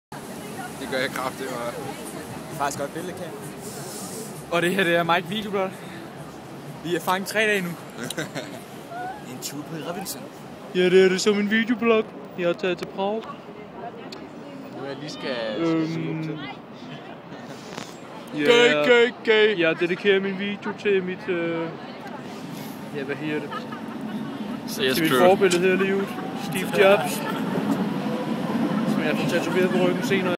iPhone 4 HD Camera Test in Prague, Charlesbridge
me filming the charles bridge in Prague, i was there few days ago.

thanks, if you want me to test anything, regarding the macbook pro "13 inch (games, or whatnot) or iPhone 4 / 3GS just contact me and i will see what i can do.